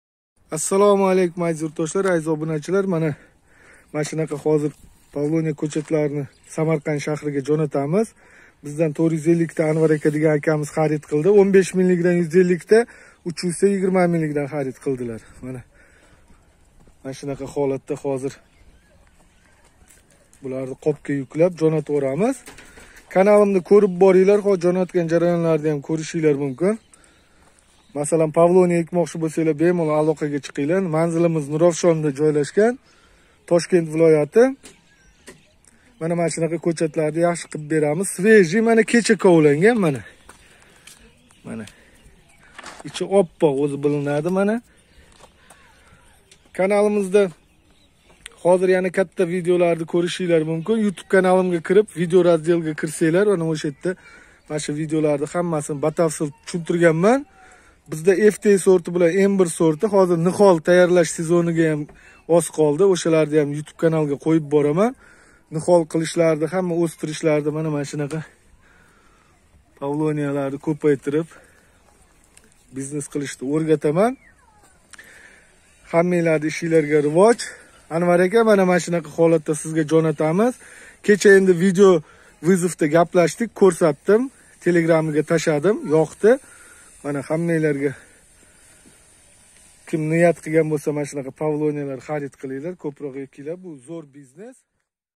Павловния Дарахти Самарканд Шахрига , тел+99890-810-18-70
дерево Павловния хорошое дерево и прибыльный бизнес план можно использовать для строительства и мебели